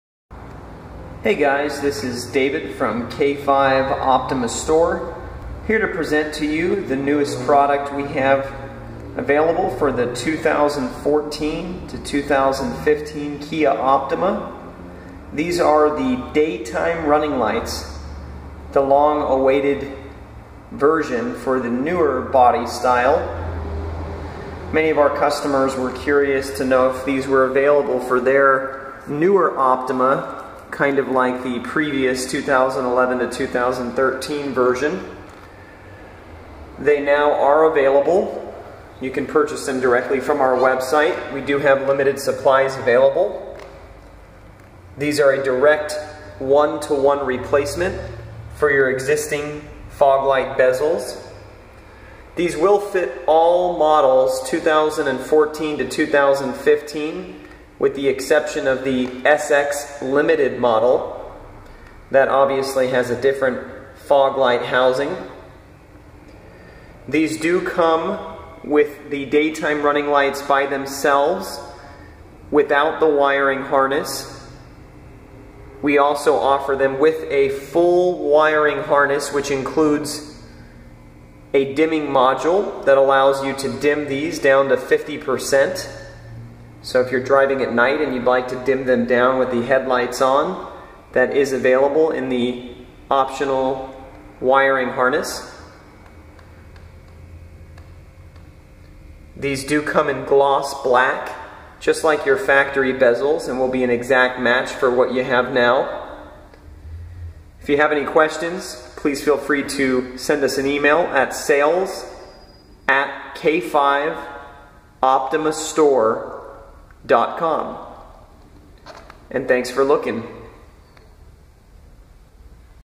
2014-2015 KIA Optima Dual Colored Daytime Running Lights
Here to present our newest product for the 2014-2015 Kia Optima.

Daytime Running Lights. These will fit all Kia Optima models with the exception of the SX-L model. These are a direct 1:1 replacement for your current setup.

There is no cutting or modifying of your OEM parts to install these. We have an optional wiring harness with dimmer module available when ordering.